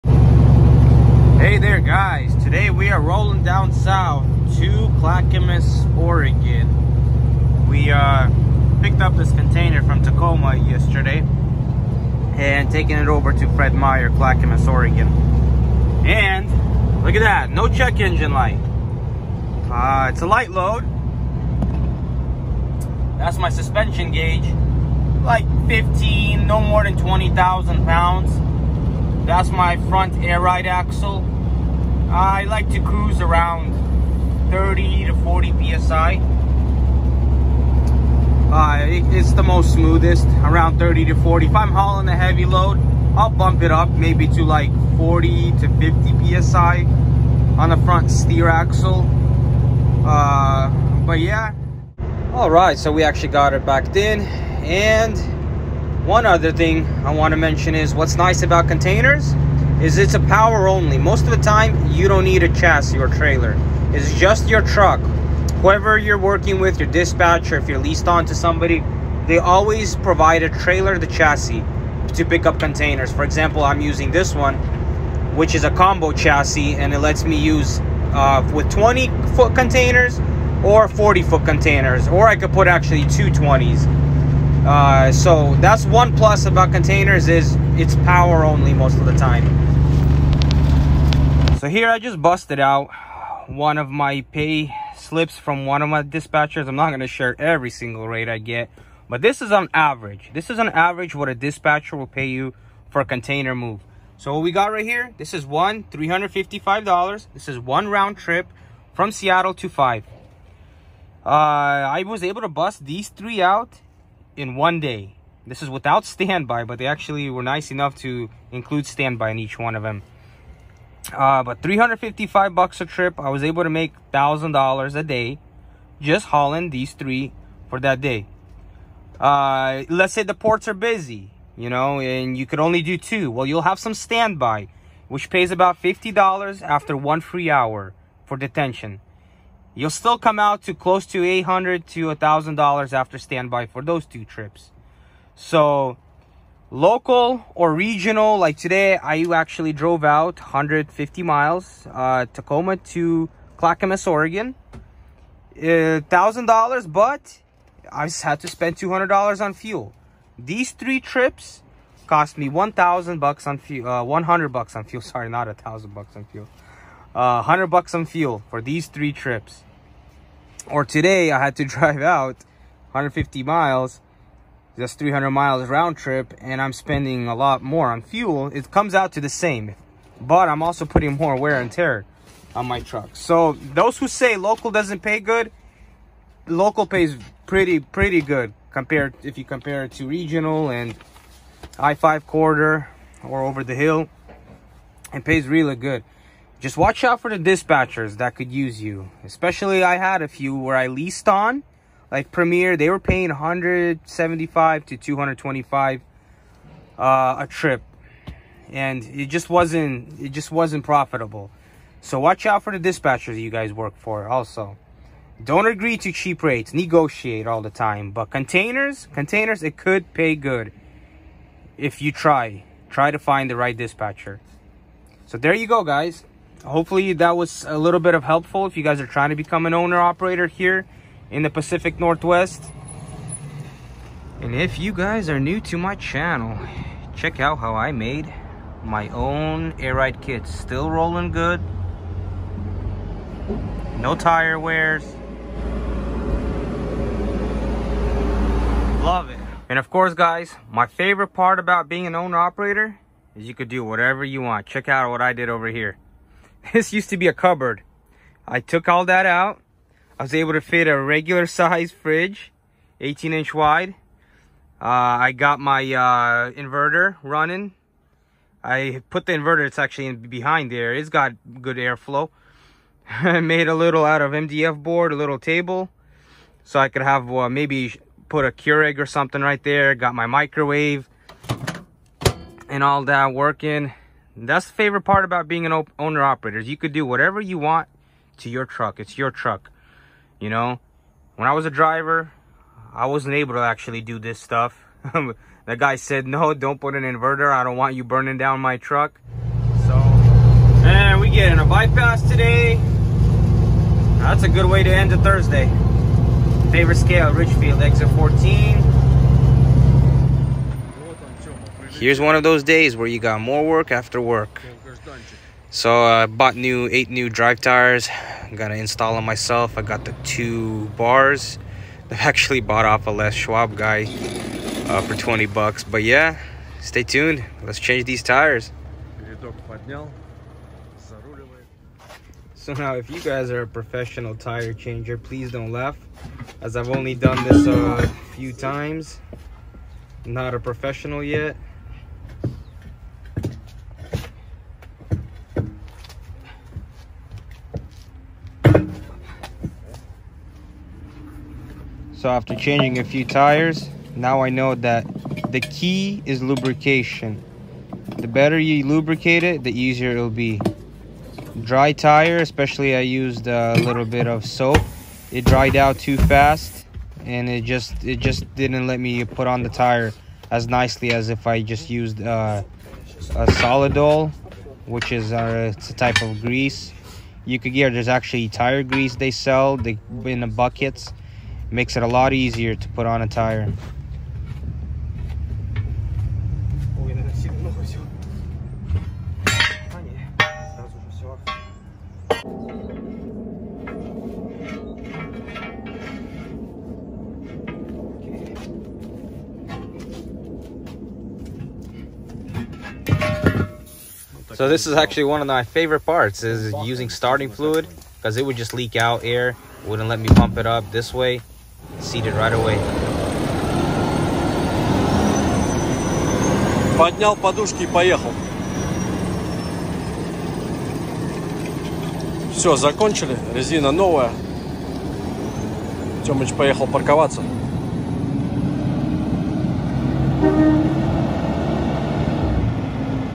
Owner operator Pay hauling containers | Is it Worth it?
Small info video on anyone interested on becoming an Owner Operator hauling containers in the PNW. Ask any question and I’ll answer them the best I can!